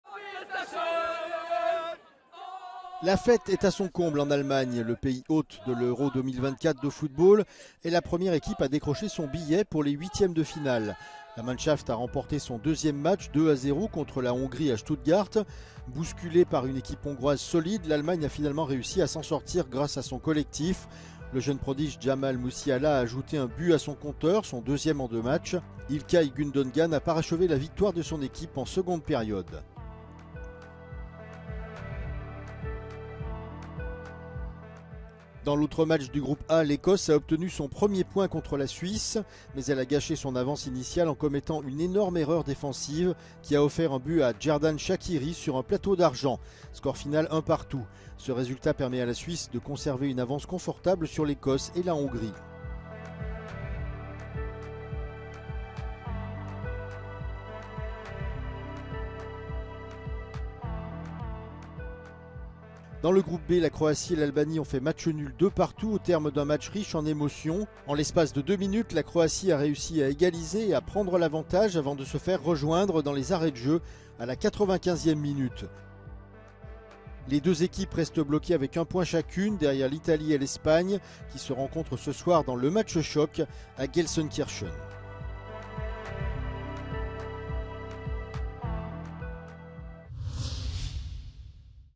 Euro 2024 : l'Allemagne domine la Hongrie, la Croatie et l'Ecosse s'éloignent des 8e de finale
L'Allemagne première qualifiée pour les 8e. La Suisse bien placée. Dans le groupe B la Croatie et les Albanais voient leur chance de qualification se réduire.